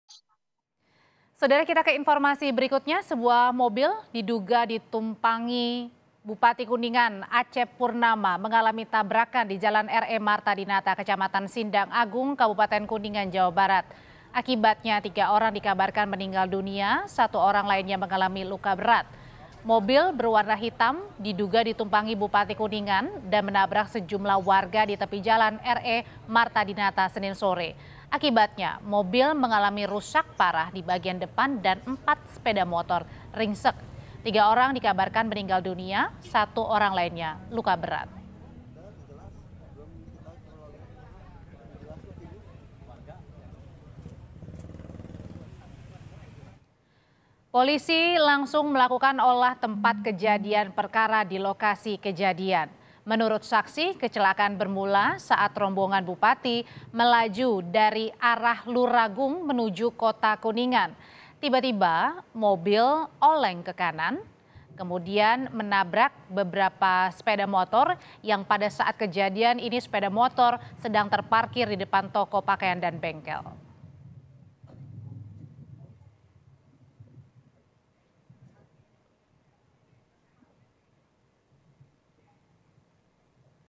Kronologi Kecelakaan Rombongan Mobil Bupati Kuningan
KUNINGAN, KOMPAS.TV - Sebuah mobil yang diduga ditumpangi Bupati Kuningan Acep Purnama mengalami tabrakan di Jalan RE Martadinata, Kecamatan Sindang Agung, Kabupaten Kuningan, Jawa Barat.

Akibatnya, 3 orang dikabarkan meninggal dunia, sementara 1 orang lainnya mengalami luka berat.

Mobil mengalami rusak parah di bagian depan dan 4 sepeda motor ringsek.

Polisi pun langsung melakukan olah TKP di lokasi.

Menurut saksi, kecelakaan bermula saat rombongan Bupati melaju dari arah Luragung menuju Kota Kuningan.

Tiba-tiba mobil oleng ke kanan dan menabrak beberapa sepeda motor yang sedang terparkir  di depan toko pakaian dan bengkel.